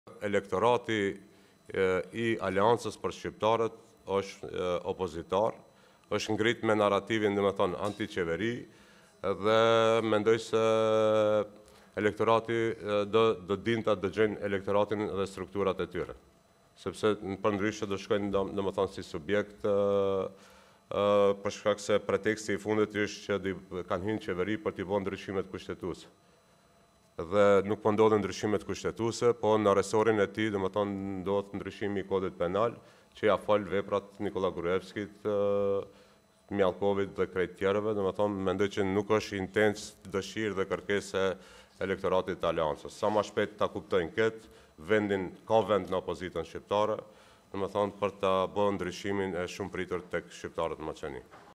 Mexhiti: Ministria e Krenar Llogës po i’a fal veprat Gruevskit dhe Mijallkovit‼
Na ndiqni:
Prodhoi:LajmTV, Nentor 2023

© Gazeta LAJM - Të gjitha të drejtat janë të rezervuara.